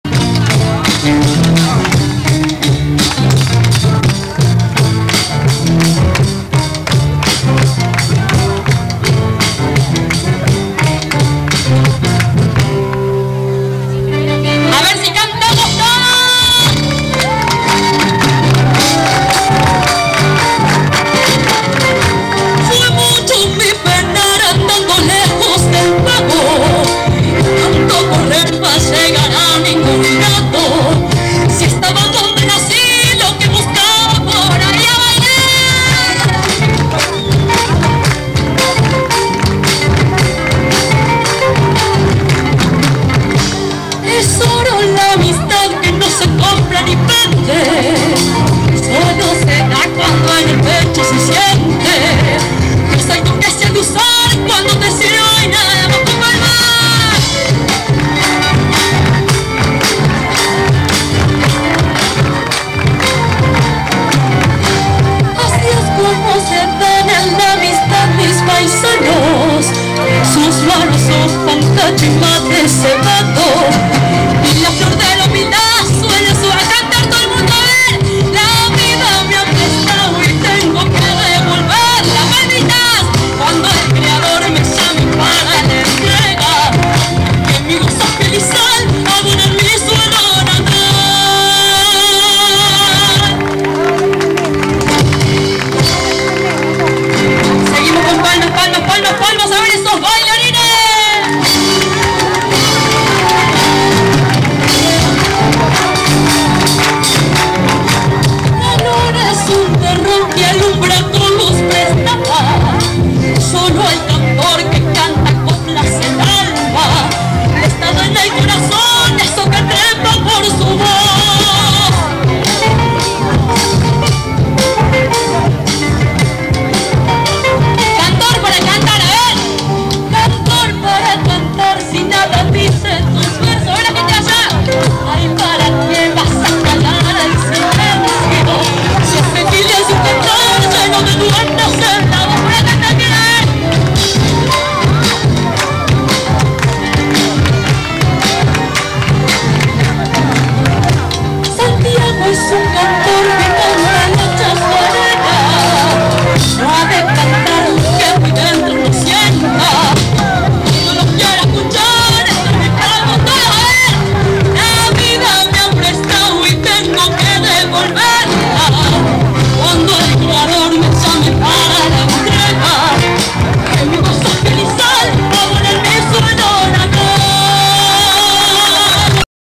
ALFONSINA -
Cantante de Villa Cura Brochero, interpretando una chacarera en los espectàculos callejeros de Cosquìn 2012.